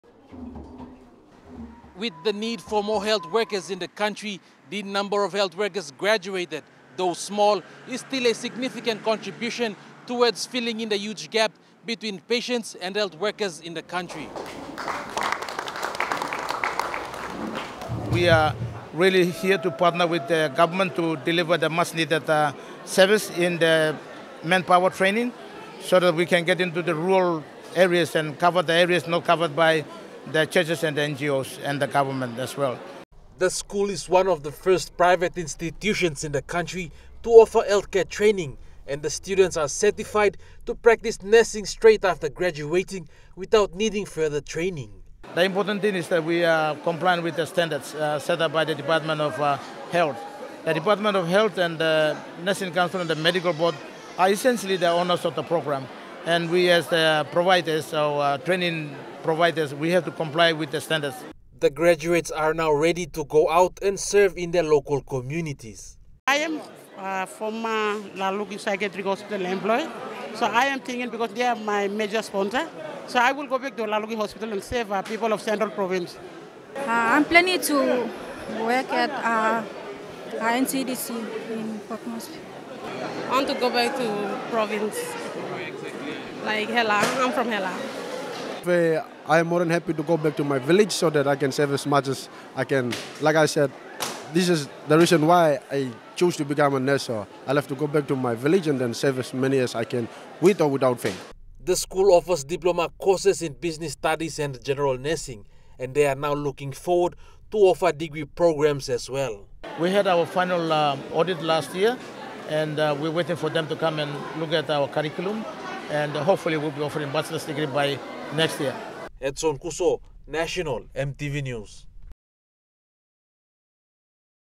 APIASETS 9TH GRADUATION
The country's ratio of nurses to patients stands at 1 nurse to 2000 patients; hence it’s a challenge for nurses to treat patients daily.
To increase the number of nurses to patients, Asia Pacific Institute of Applied Social, Economic & Technical Studies graduated 36 health workers.